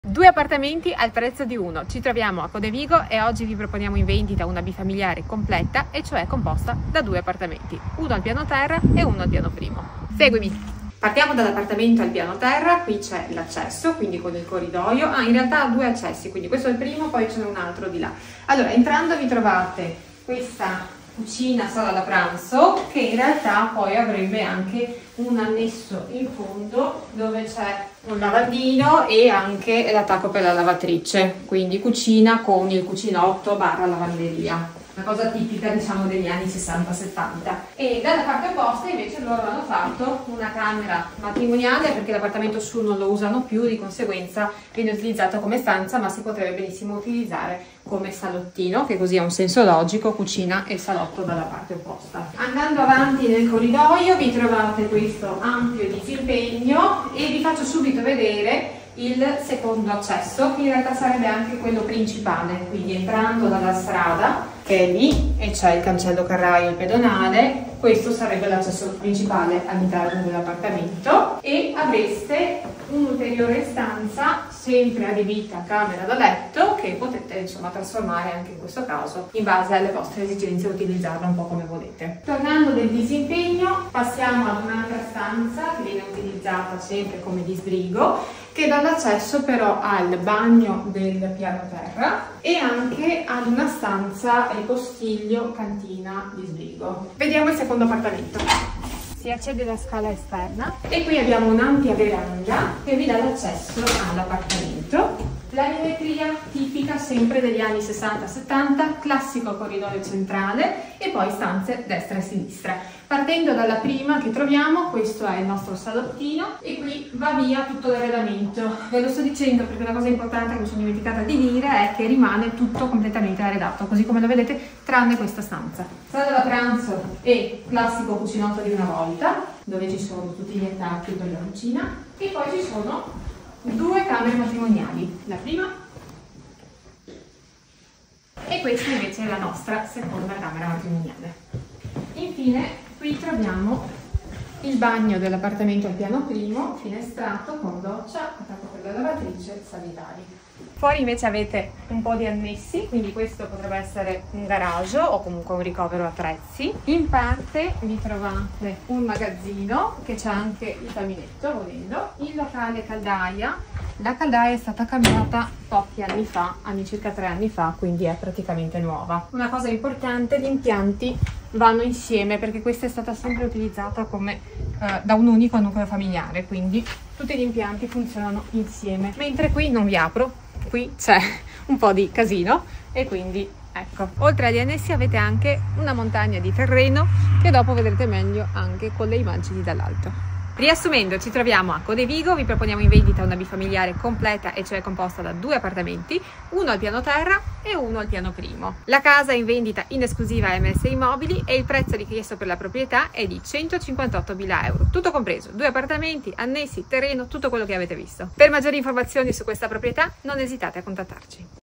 Bifamiliare completa in vendita a Codevigo
In vendita una bifamiliare situata nel comune di Codevigo, ideale per due nuclei familiari grazie alla sua suddivisione in due appartamenti distinti. Costruita negli anni ‘60, la casa presenta finiture originali dell’epoca ma è abitabile da subito, nel senso che si può entrare e vivere senza necessità di interventi strutturali immediati.

L’appartamento al piano terra, con una superficie di circa 94 mq, è composto da una sala da pranzo con cucinotto, due camere matrimoniali, un disimpegno, un locale disbrigo, un bagno con doccia e un ripostiglio. L’appartamento ha due accessi separati e un’altezza inferiore a 2,70 metri, che non incide sul comfort abitativo, rendendolo comunque accogliente e funzionale.

L’appartamento al piano primo, accessibile tramite una scala esterna indipendente, ha una superficie di circa 114 mq e offre spazi più ampi, con una veranda, un ampio disimpegno, due camere matrimoniali, un salotto, una sala da pranzo con cucinotto e un bagno.

Gli impianti sono condivisi tra i due appartamenti: c’è un’unica caldaia per l’intera proprietà e un unico contatore della luce, poiché la casa è stata utilizzata dalla stessa famiglia nel corso degli anni.

La proprietà è circondata da un ampio giardino privato di 1650 mq, di cui 650 mq sono censiti come vigneto. Completano la proprietà vari annessi, tra cui un ricovero attrezzi, un magazzino, un locale caldaia e un ulteriore magazzino, ideali per chi cerca spazi esterni aggiuntivi.

Questa bifamiliare è perfetta per chi desidera una soluzione indipendente e spaziosa, pronta per essere vissuta. È importante notare che, in caso di acquisto da parte della stessa persona, solo uno dei due appartamenti potrà essere acquistato con mutuo e agevolazioni prima casa. Se due persone diverse acquistano gli appartamenti, entrambe potranno beneficiare del mutuo e delle agevolazioni prima casa. Inoltre, la parte del giardino censita come vigneto non può essere inclusa nel mutuo e dovrà essere acquistata con fondi propri.

La consegna della casa è prevista entro 6-8 mesi dal preliminare, per permettere ai venditori di trovare una nuova abitazione. Una rara opportunità per chi cerca una casa con ampi spazi, indipendenza e tranquillità in un contesto immerso nel verde.

MS Immobili
Via Zago 101, 35020 Saonara (PD)